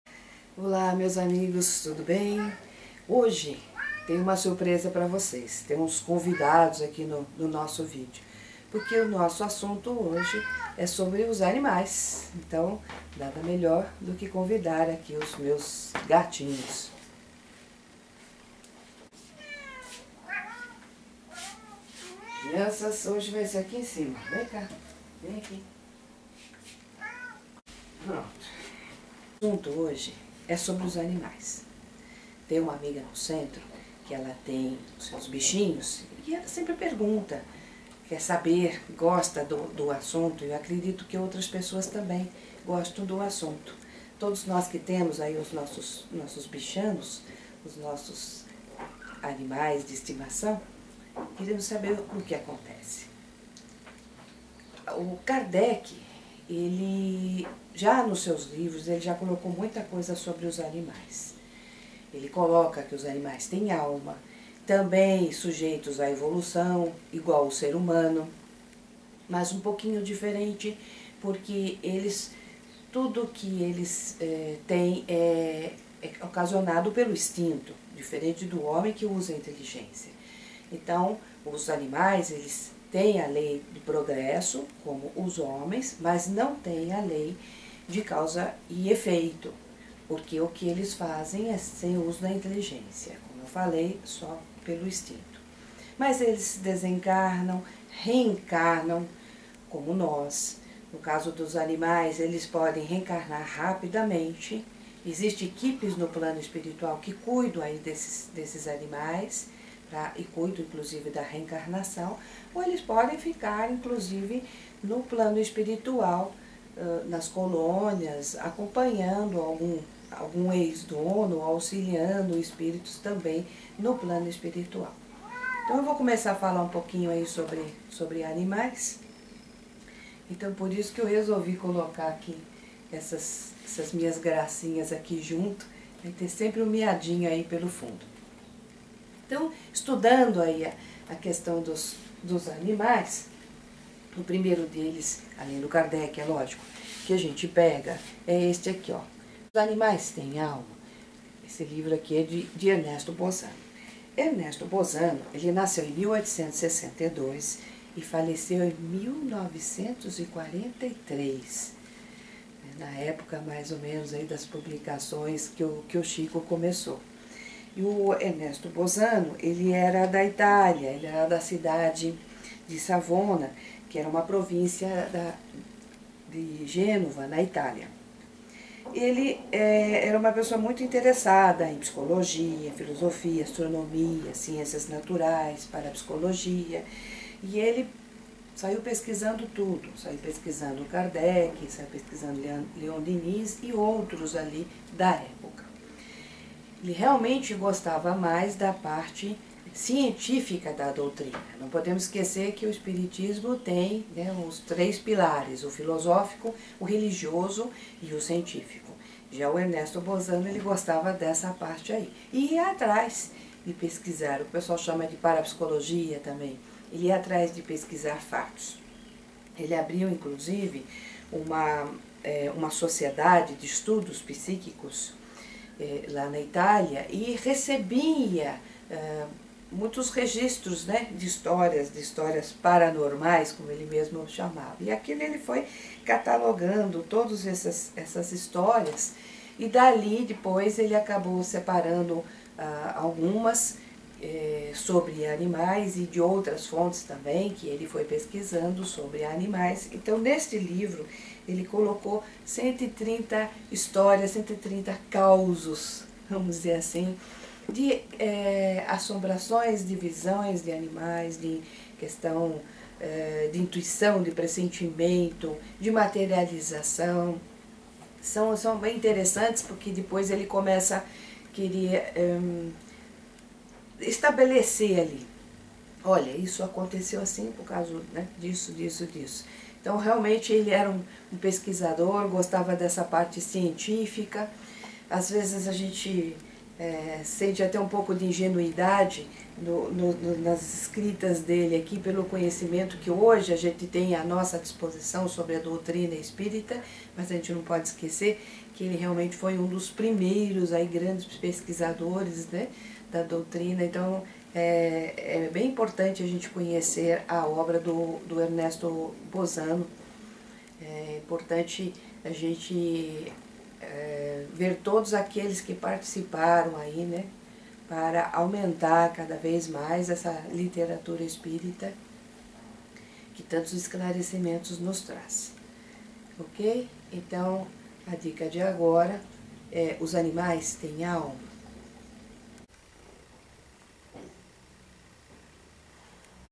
Os Animais Têm Alma - Ernesto Bozzano
Ernesto Bozzano ficou conhecido como o mestre da alma, devido as suas pesquisas. Nasceu em 1862 na cidade de Savona, província de Gênova na Itália e desencarnou em 1943. Interessava-se por filosofia, psicologia, astronomia, ciências naturais e  parapsicologia. Além disso investigou sobre a ciência da alma, defendeu a tese espírita e fundou a Sociedade de Estudos Psíquicos: O Círculo Científico Minerva, onde foram registradas manifestações de toda espécie. Desses registros publicou 130  neste livro, apenas sobre animais. Autor espírita clássico.